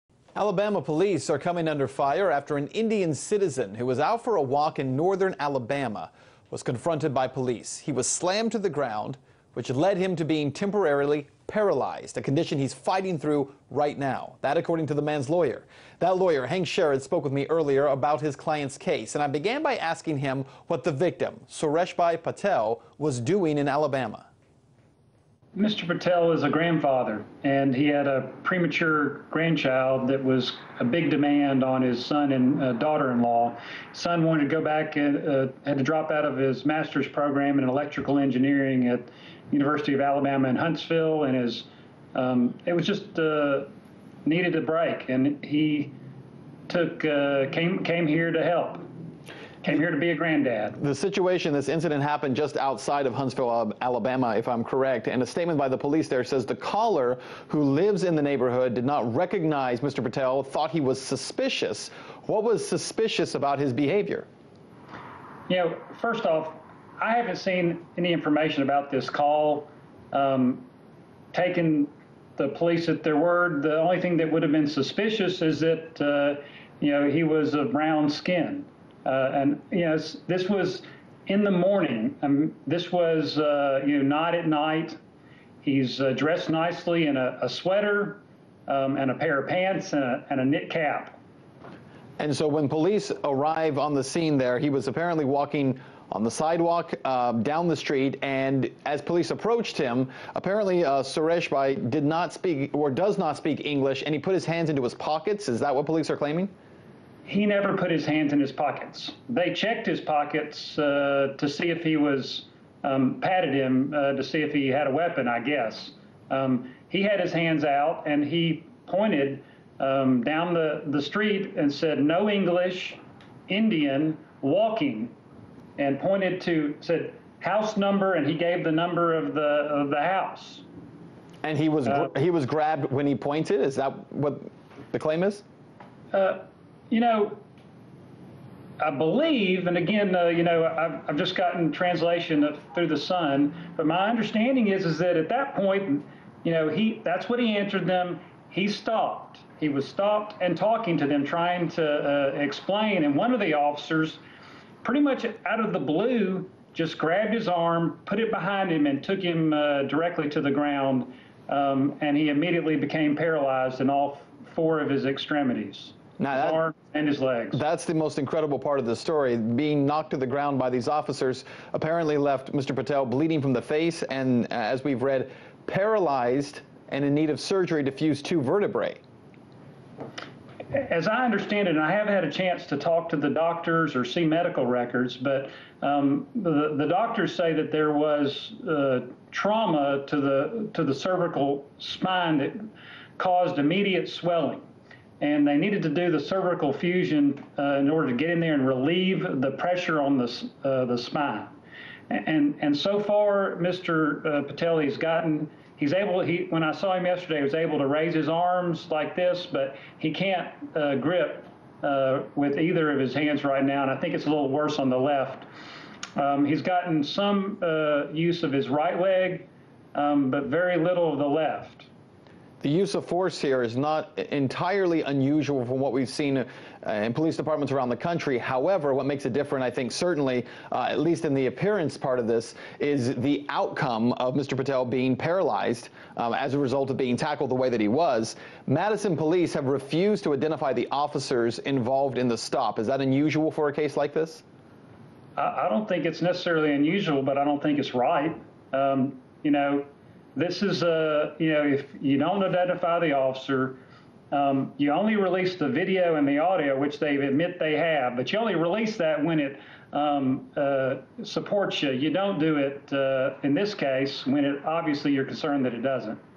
Horror of Alabama police | American News
American News Police in Alabama have been accused of temporarily paralyzing an Indian man who was helping his son's family care for their premature baby. “One of the officers, grabbed his arm, took it.

A man from India was beaten up by Alabama police while walking near his son's home merely for having brown skin, his lawyer says. Sureshbhai Patel, 57, was left temporarily paralyzed and.

India Asks US To Probe Reports Of Police Brutality On Grandfather India Asks US to Probe Reports of Police Brutality on Grandfather The government has asked the United States to urgently.